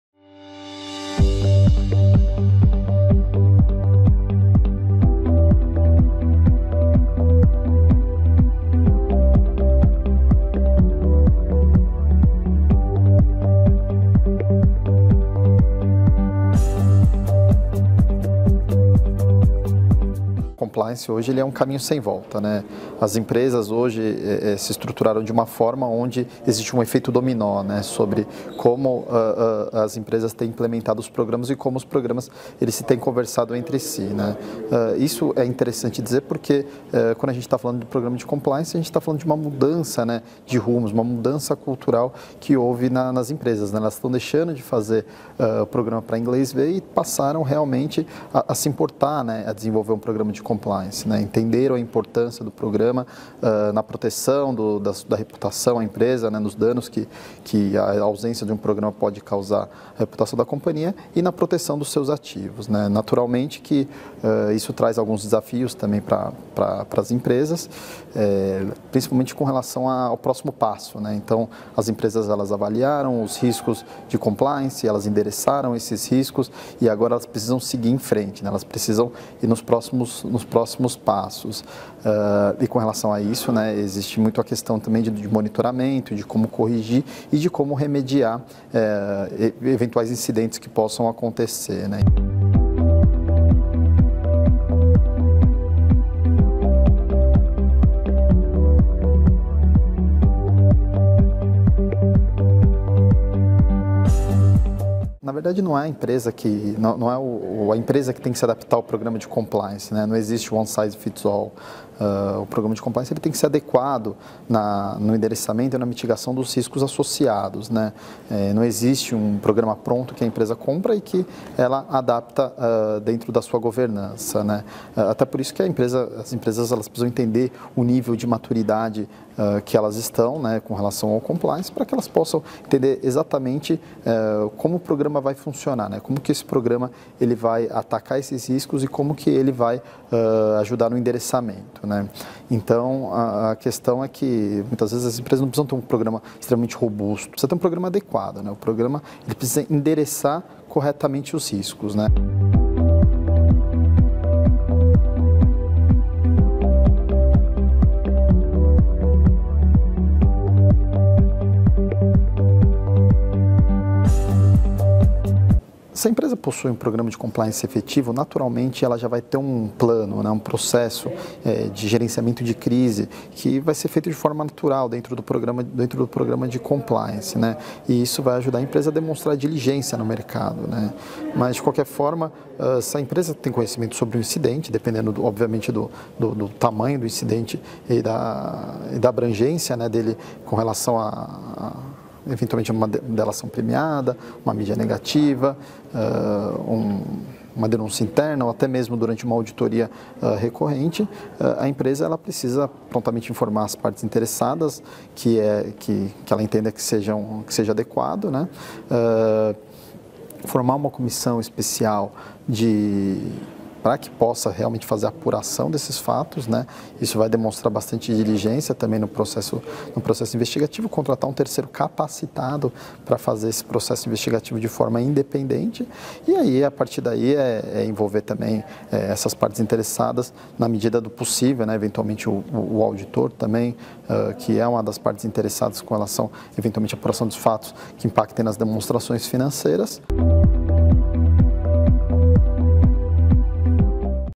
Programa de Compliance | Vitor Pedrozo, diretor da área de FIDS da Grant Thornton Brasil
Programa de compliance está relacionado a uma mudança cultural das empresas, em que a proteção da reputação e dos ativos da companhia estão cada vez mais em evidência. Nesta entrevista concedida à EBDI e veiculada no canal da Harvard Business Review Brasil, o diretor da área de FIDS da Grant Thornton Brasil, Vitor Pedrozo, fala sobre as vantagens e os desafios de adequar a governança corporativa ao programa de compliance. Confira!